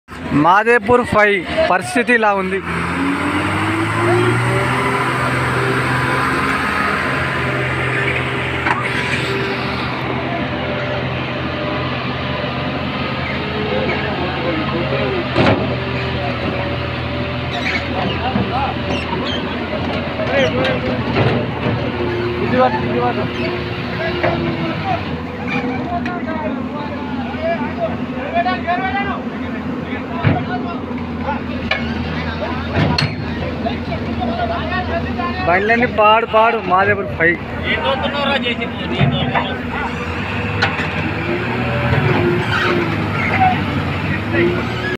SAND LODE 12 TYRE LARRY STUK IN SAND GET HELPING Jcb
All mecheins video post our channel see enjoy